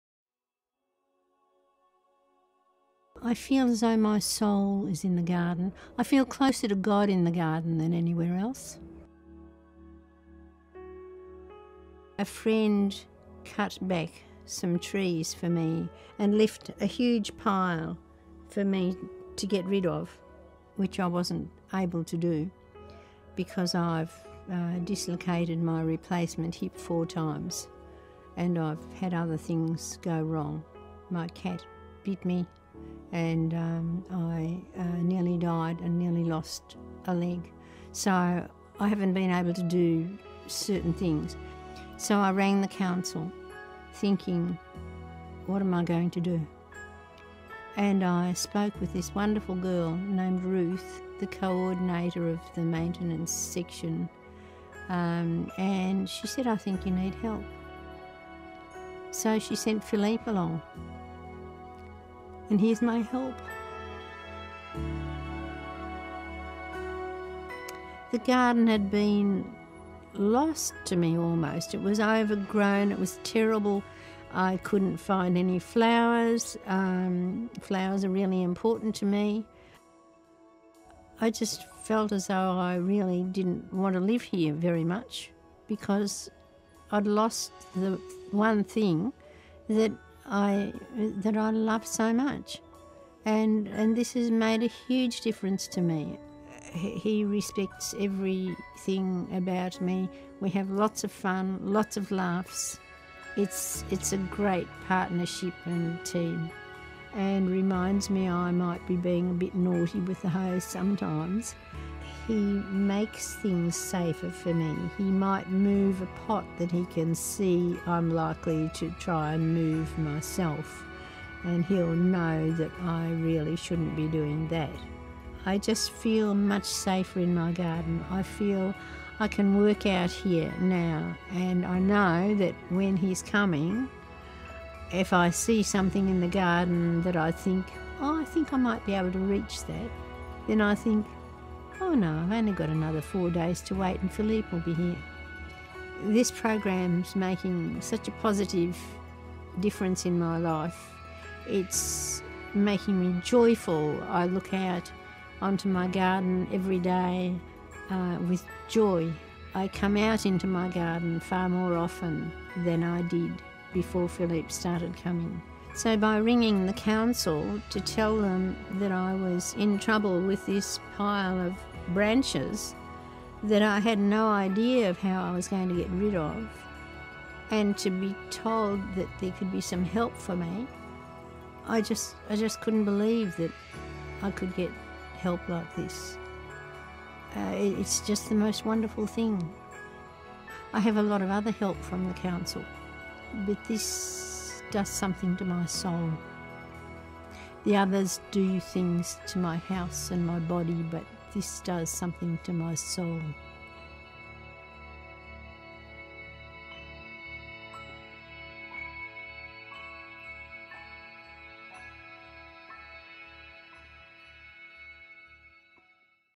City of Greater Dandenong | Community Care
Elizabeth has found a new lease on life after Council paired her with Community Care maintenance worker Philipe. Following some health issues, Elizabeth was unable to continue looking after the garden on her own. Her garden was almost lost to her, until Philipe came in and helped her restore some order. His ongoing support has helped Elizabeth reconnect to a pastime that has a positive influence and has returned joy to her life.

Council’s Community Care program is a support service to help older residents and residents with a disability remain active in their home and community.